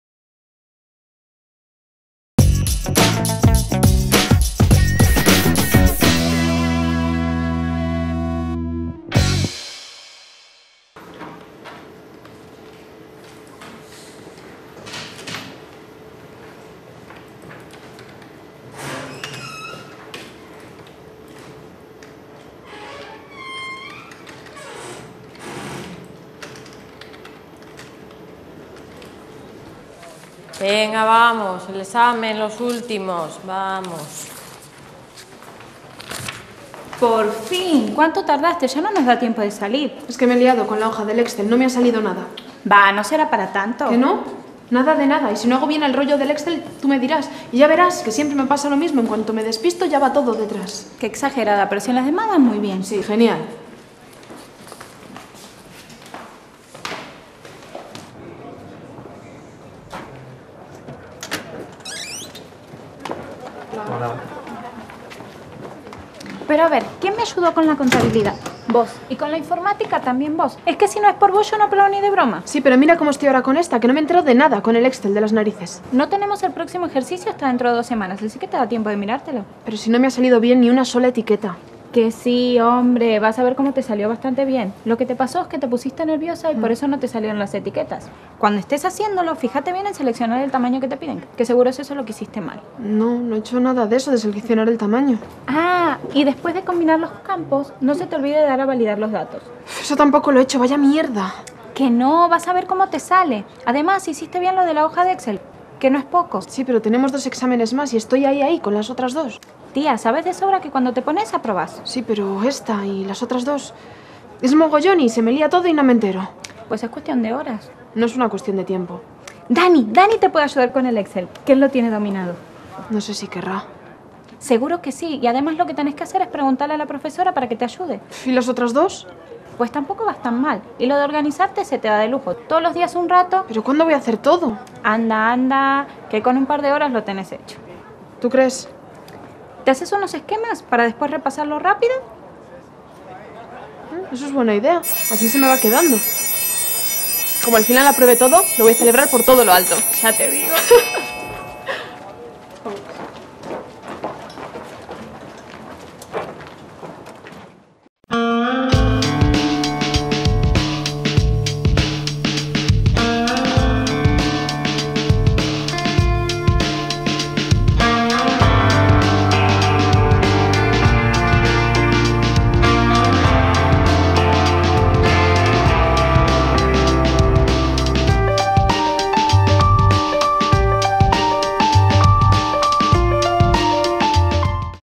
Ayuda positiva. Programa de prevención selectiva del consumo de drogas GALILEI
Video para el profesorado y educadores del Programa Galilei para la sesión 2 “creando grupo” en el que se representa como ayudar a una compañera, destacando lo positivo y sugiriendo alternativas.